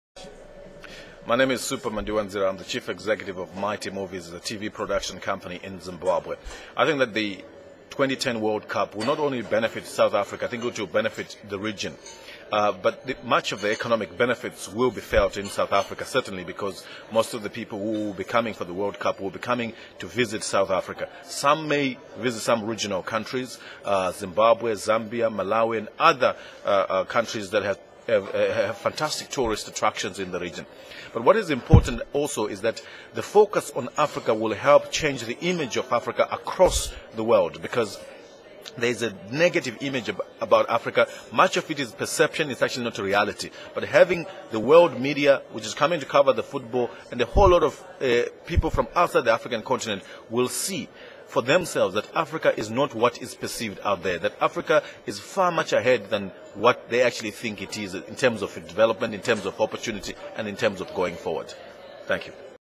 Supa Mandiwanzira - Africa Davos Debates 2009
Supa Mandiwanzira, CEO Mighty Movies, Zimbabwe, joins the the Davos Debates at the World Economic Forum on Africa held in Cape Town 10-12 June 2009